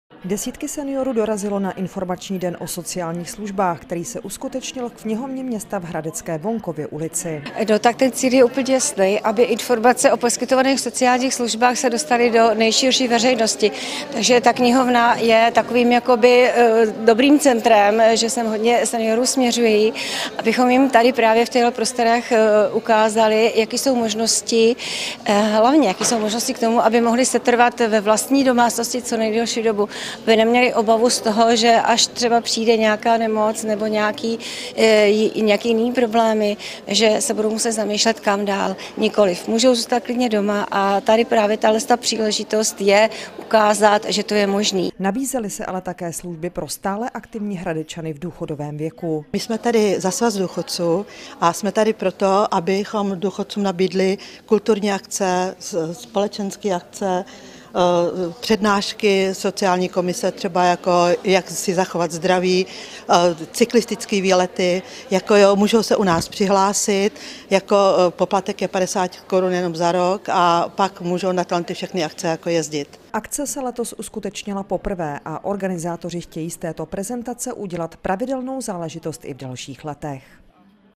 Informační den pro seniory
Desítky seniorů dorazilo na Informační den o sociálních službách, který se uskutečnil v Knihovně města v hradecké Wonkově ulici. Představily se tady organizace nabízející sociální služby, jenž v mnoha případech pomohou seniorům i přes zdravotní komplikace setrvat ve vlastní domácnosti. Akce se uskutečnila letos poprvé.